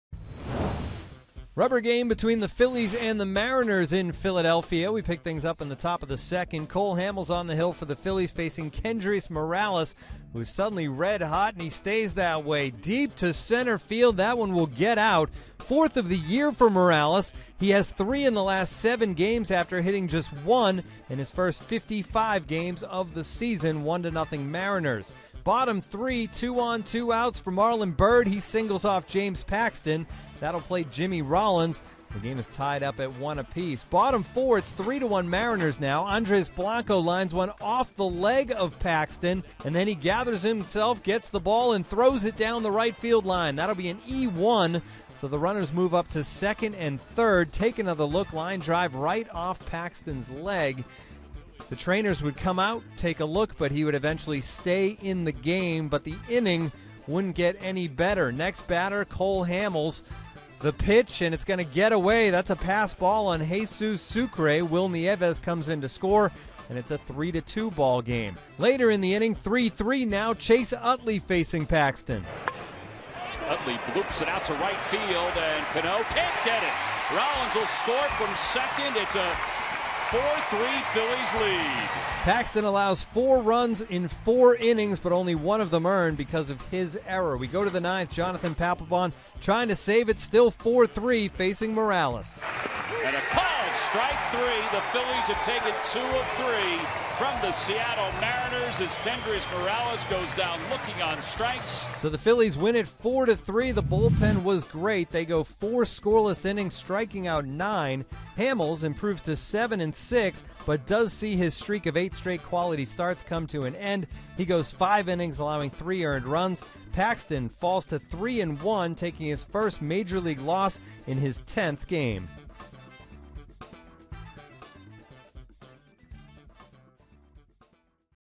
8/20/14: Phils bullpen steals show, seals win
Daily Recap: After Chase Utley's go-ahead single in the 4th, the Phillies' bullpen closed the door and Jonathan Papelbon secured a 4-3 win